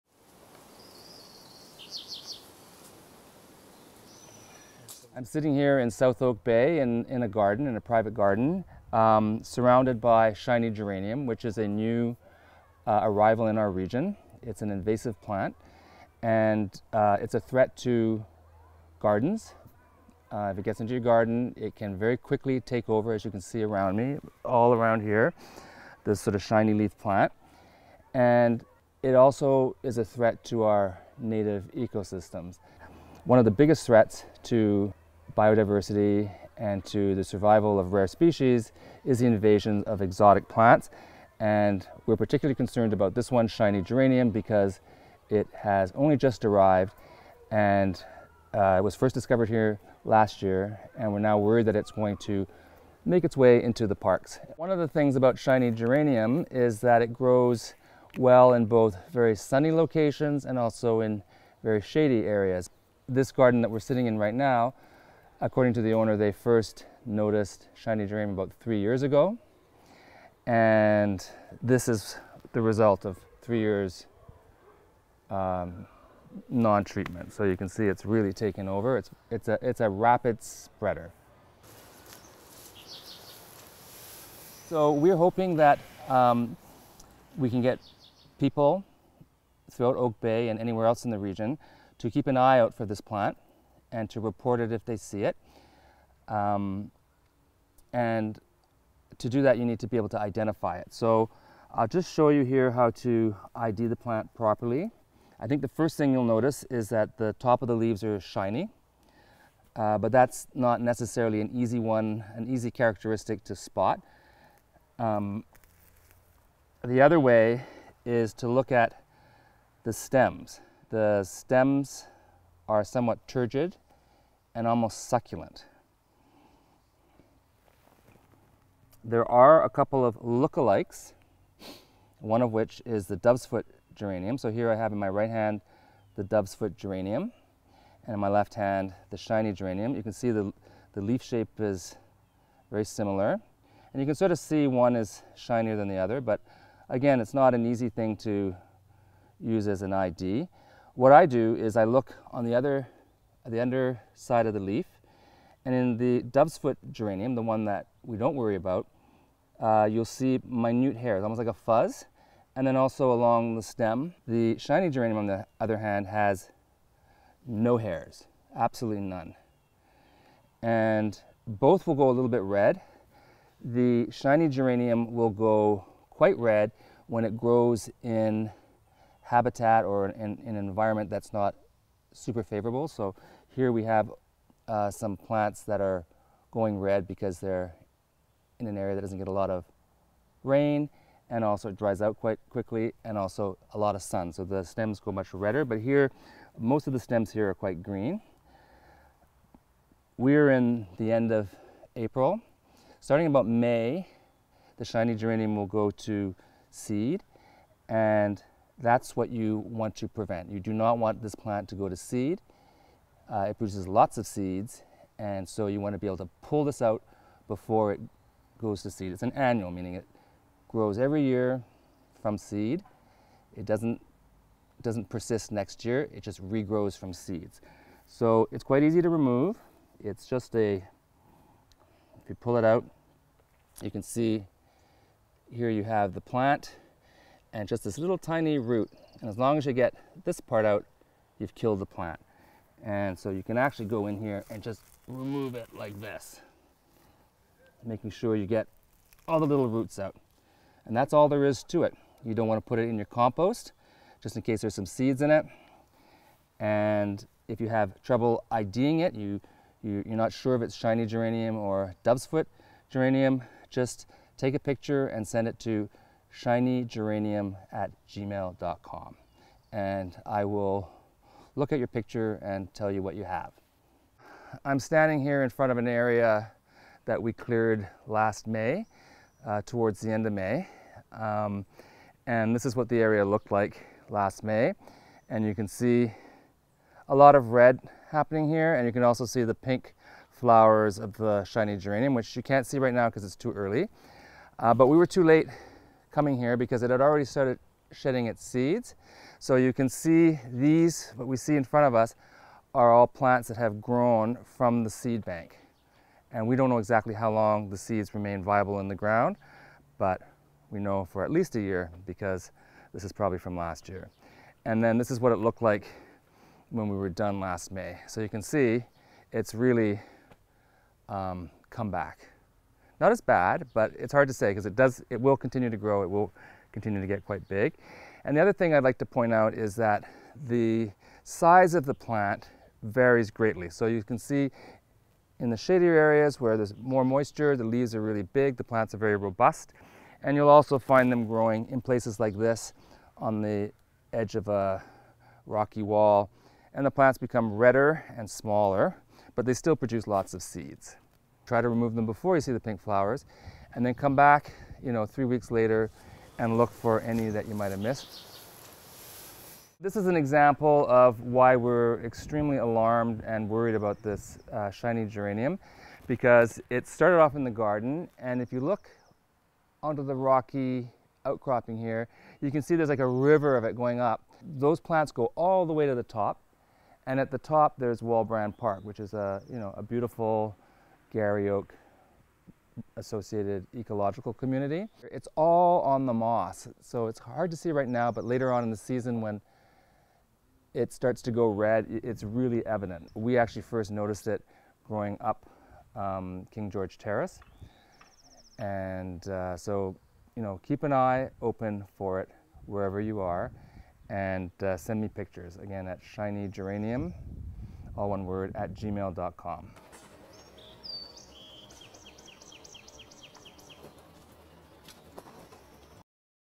Invasive Shiny Geranium
Shiny Geranium is an extremely invasive plant that is newly arrived on Vancouver Island. For more detailed information on how to report occurrences of the plant and dispose of it, please see:
1. CRD Invasive Species Alert
2. BC Provincial Invasive Species Alert